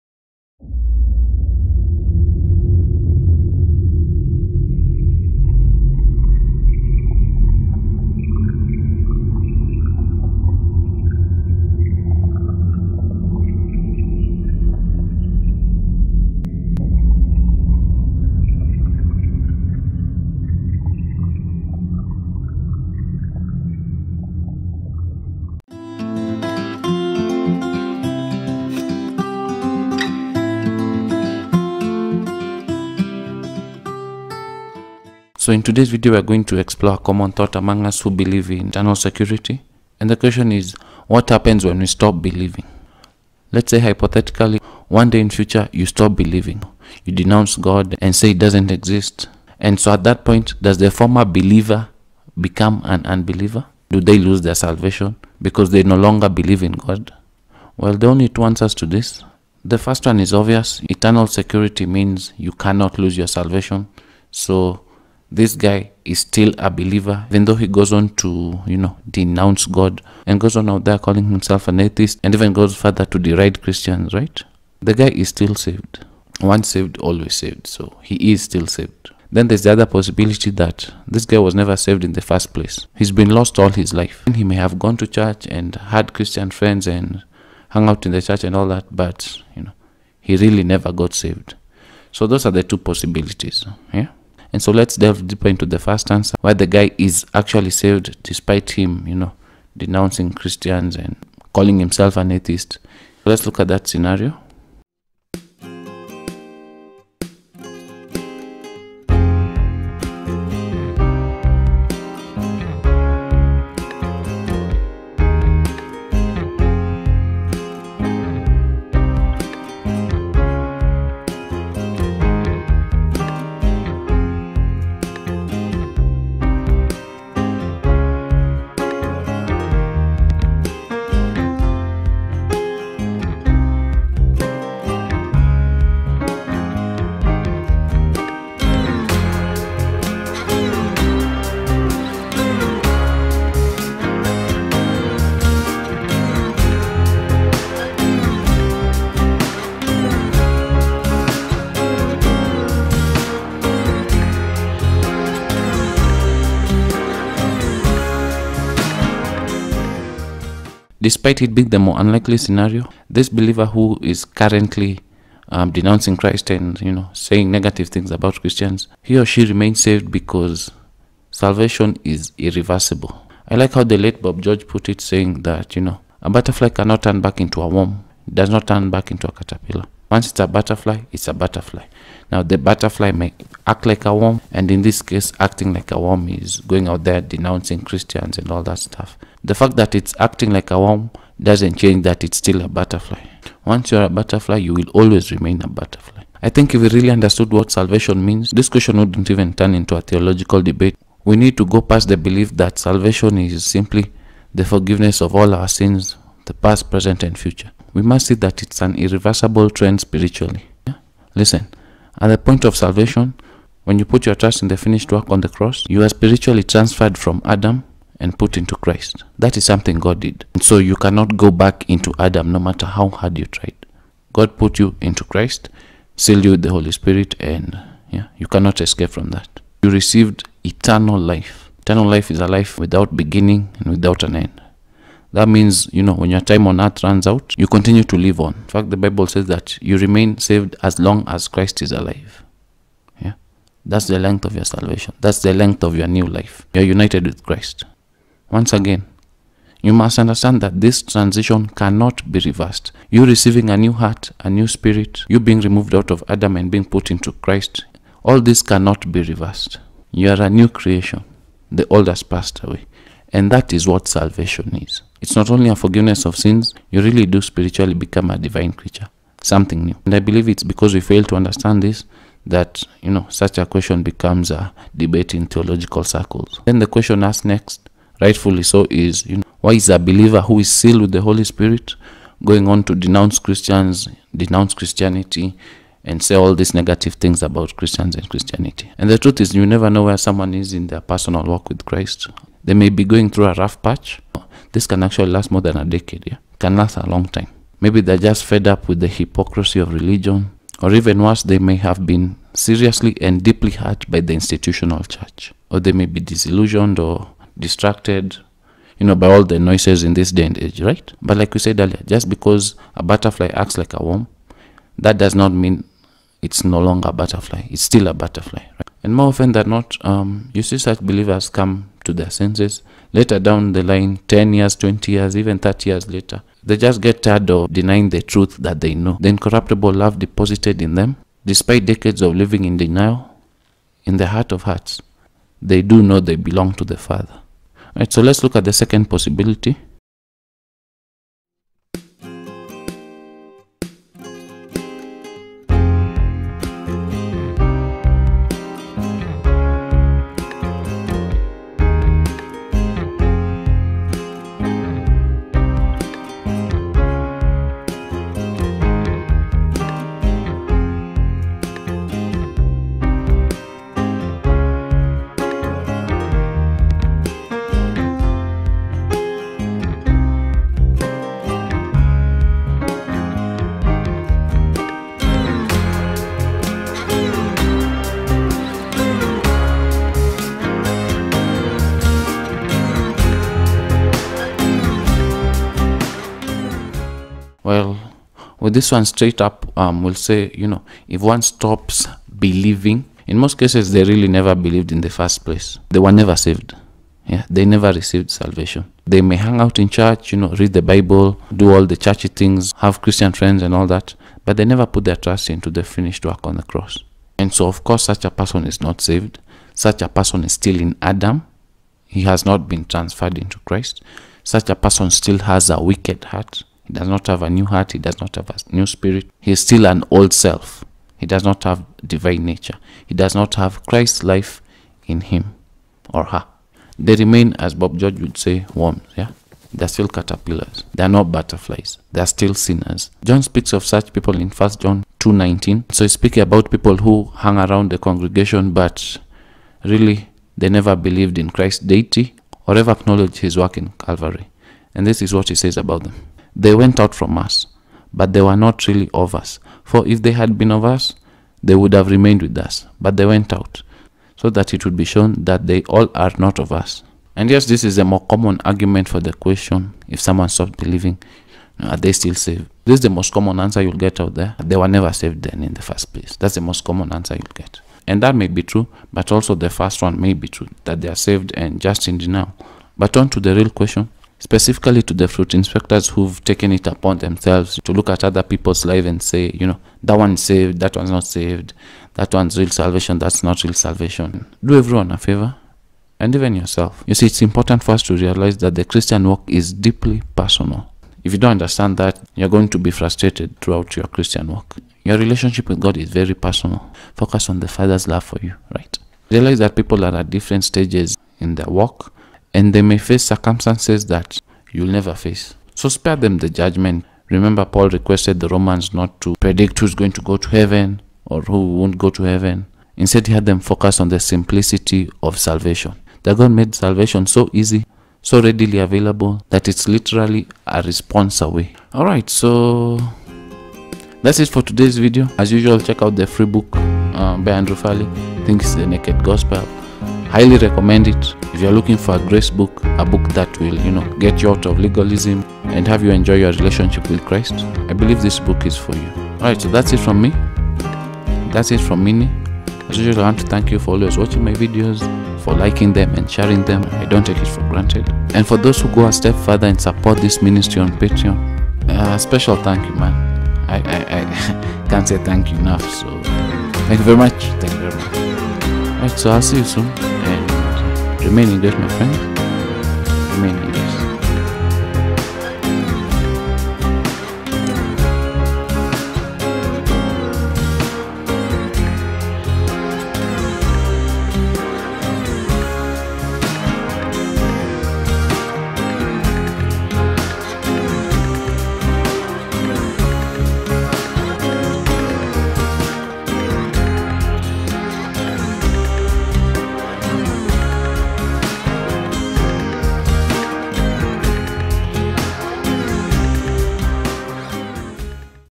Are we still saved if we stop believing?
In today's video we look at a common question we come across as proponents of the gospel of grace. Can we lose our salvation ifwe stop believing.
Link to the free downloadable grace book.